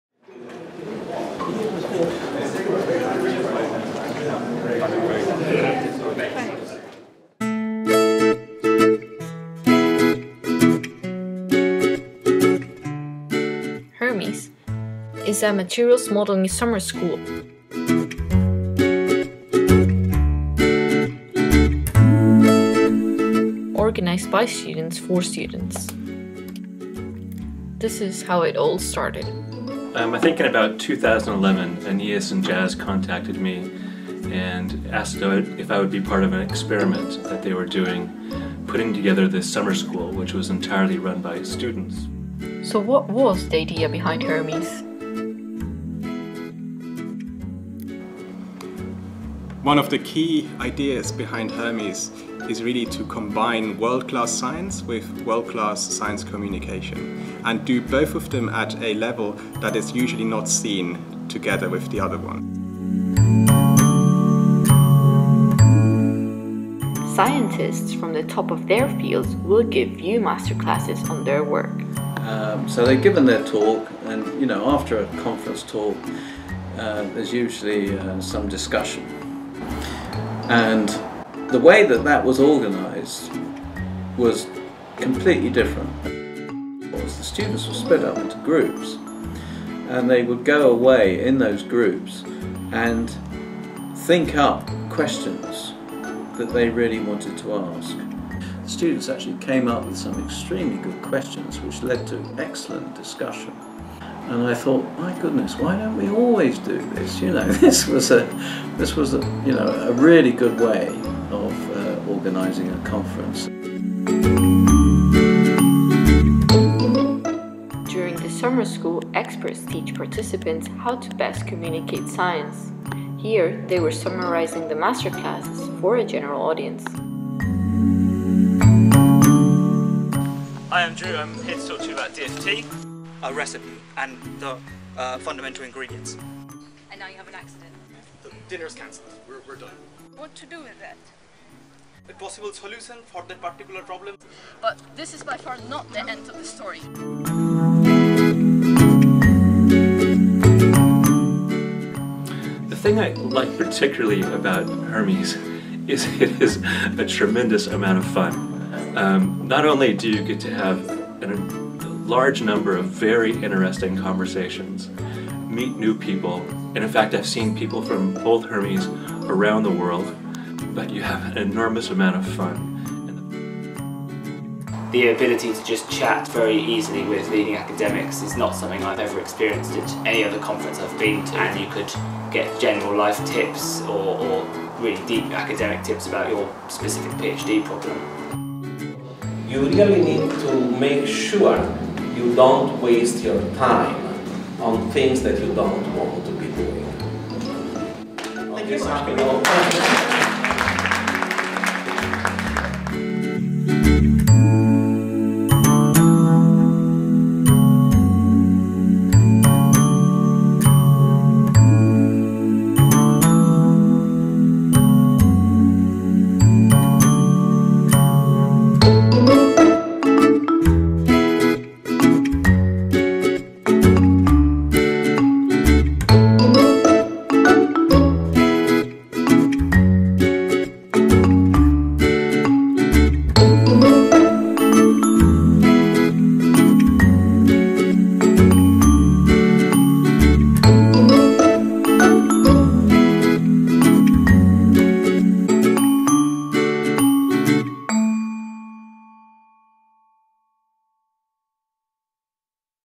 Hermes Summer School
Hermes is an international summer school committed to excellence in materials modelling and science communication. The interdisciplinary summer school is organised by PhD students from a variety of different institutions. Each of them previously attended a Hermes summer school and were inspired to build upon them to provide the opportunities to a new cohort. Continuing in the spirit of the highly successful Hermes 2012 and Hermes 2014 conferences, Hermes 2016 promises to provide an enriching experience for all participants.

Hermes 2016 will be held 27th - 31st July at Cumberland Lodge.